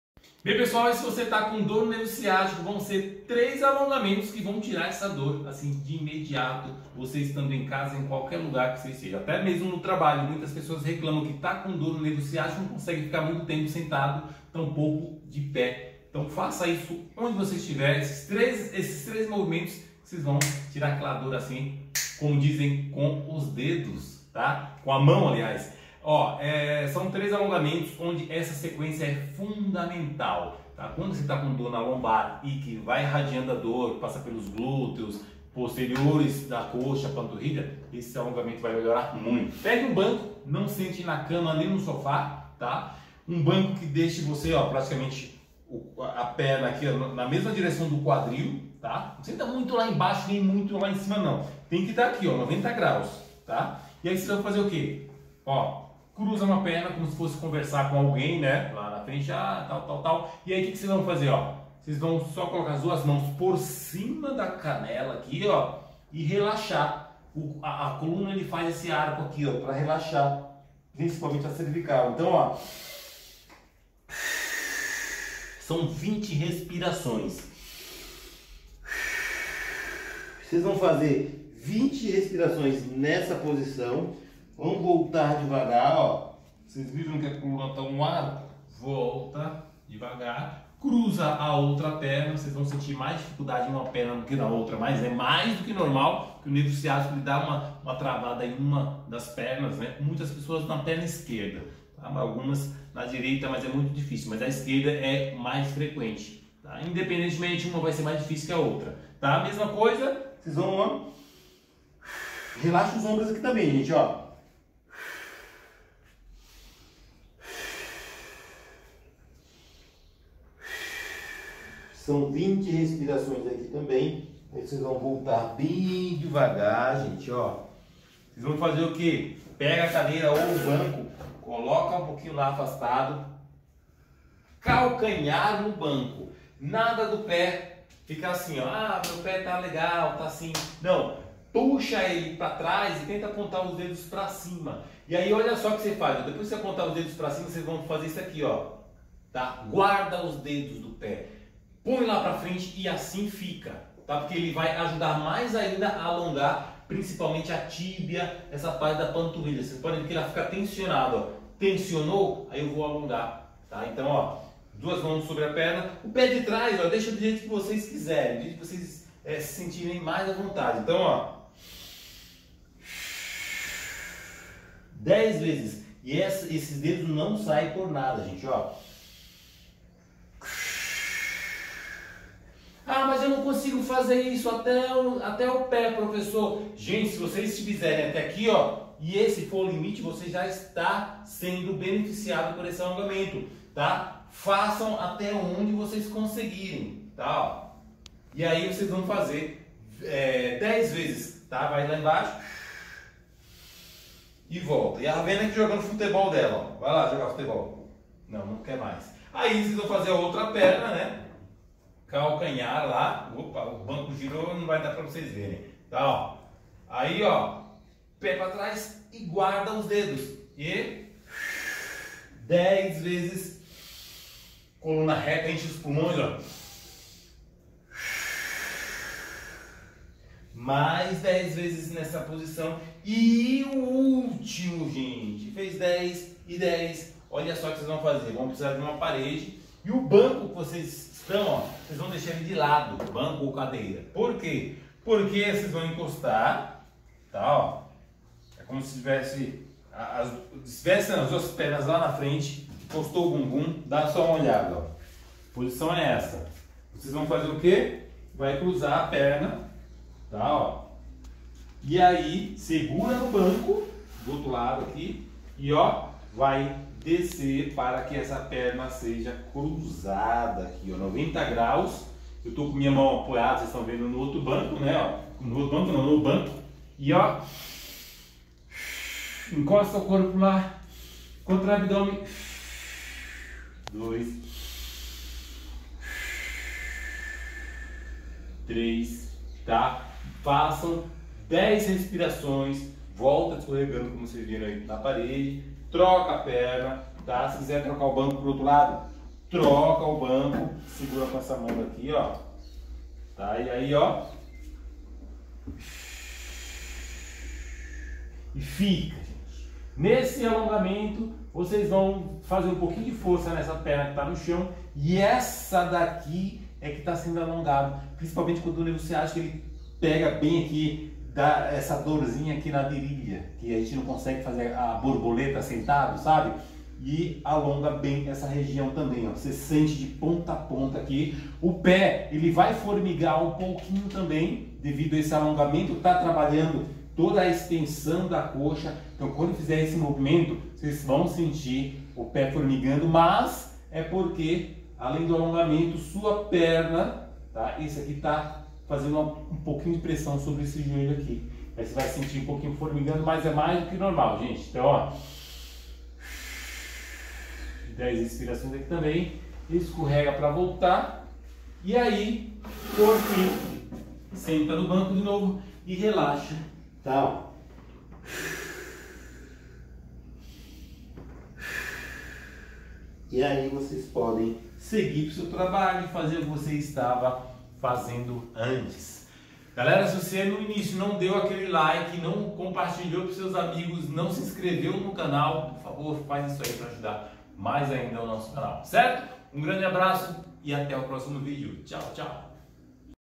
como tirar a dor do CIÁTICO e da HÉRNIA DE DISCO
Melhor alongamento para tirar sua dor da lombar e do nervo ciático quando esta atacado. Faça esse alongamento todos os dias e vai deixar seu nervo ciático relaxado.